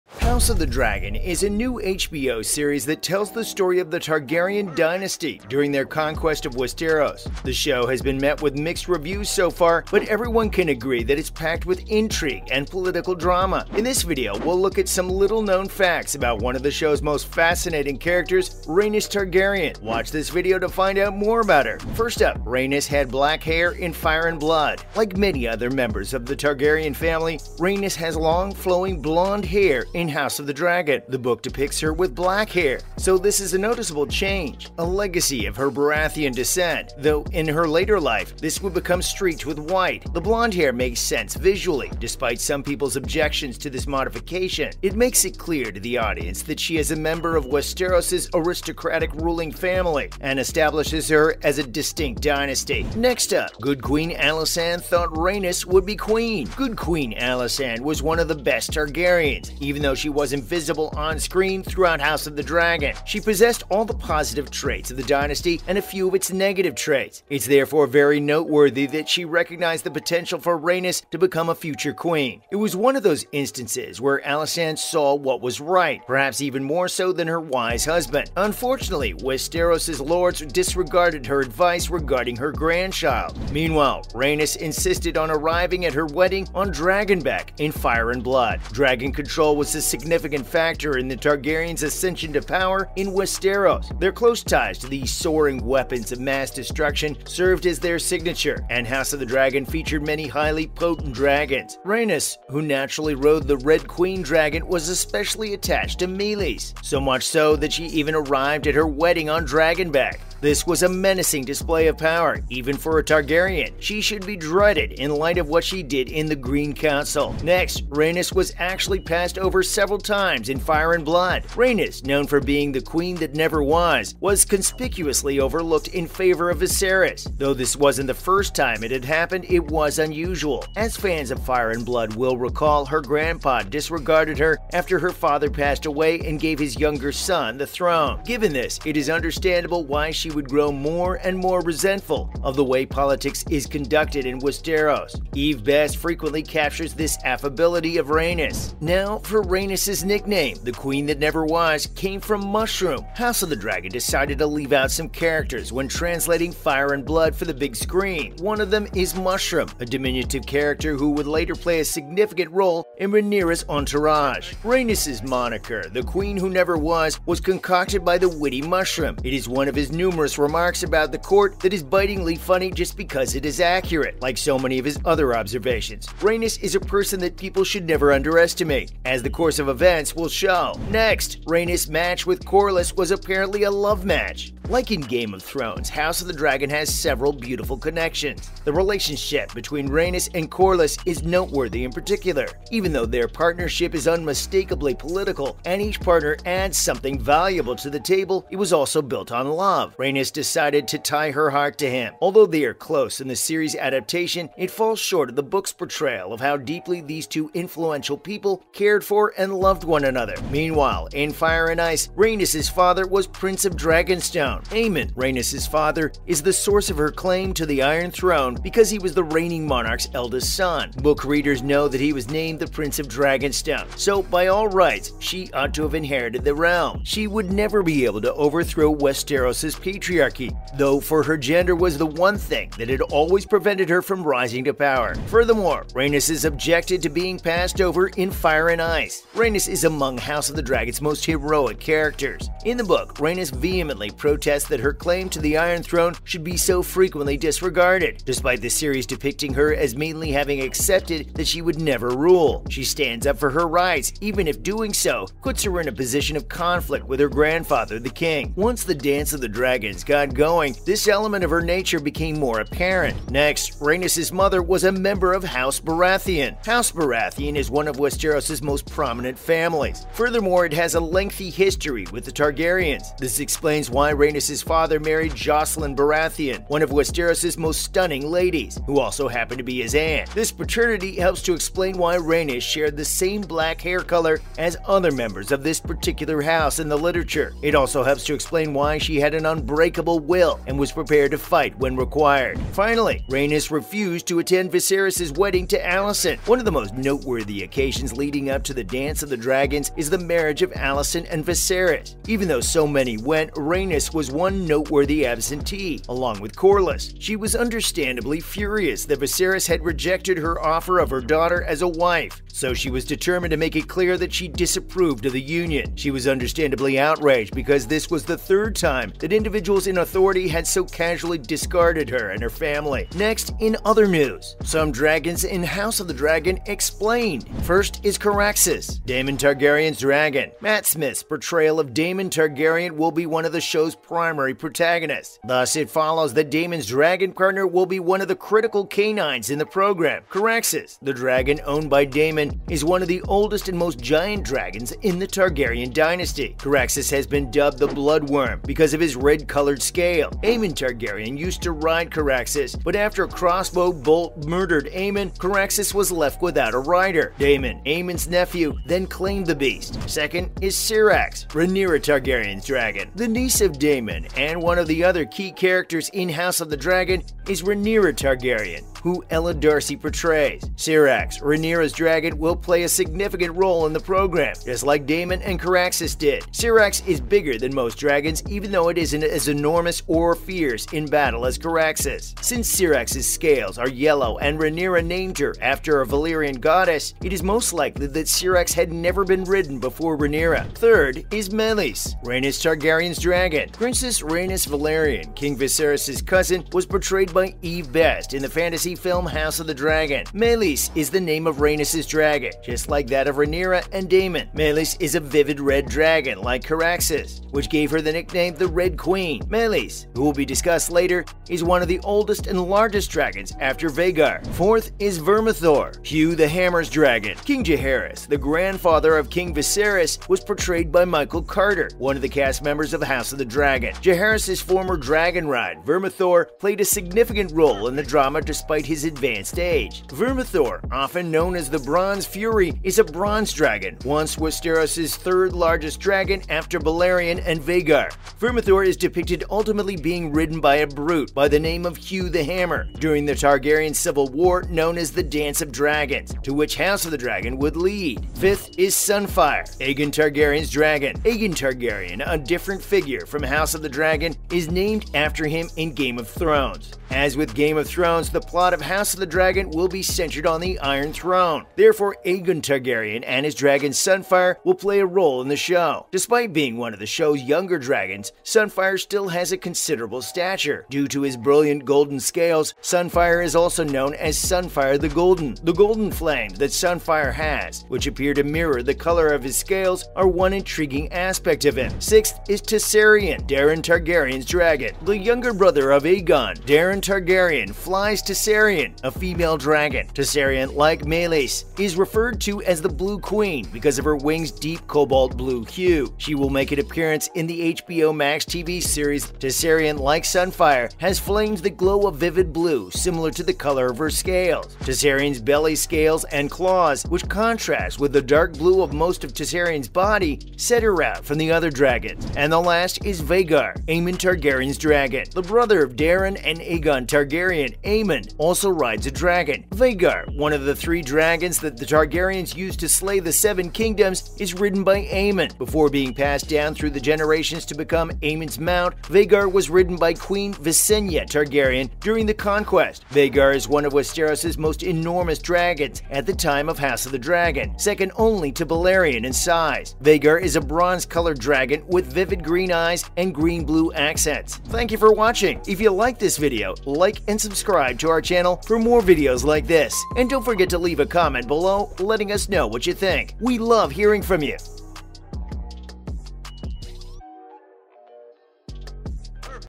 House Of The Dragon: UNKNOWN Facts About Rhaenys Targaryen..
Welcome back to Show Scope. In the House of the Dragon episode, titled “The Green Council,” Eve Best turns in a powerful and scene-stealing performance as Rhaenys, The Queen that Never Was. Although she has hovered at the edges of the story from the very beginning, when her claim to the Iron Throne was overlooked in favor of that of Viserys, only now does she truly emerge into the light.

Of course, this is no surprise to readers of Fire & Blood. Indeed, there are many things that book readers know about Rhaenys that allow them to have a deep appreciation for her as a character. Here are House of the Dragon book facts you didn’t know about Rhaenys Targaryen.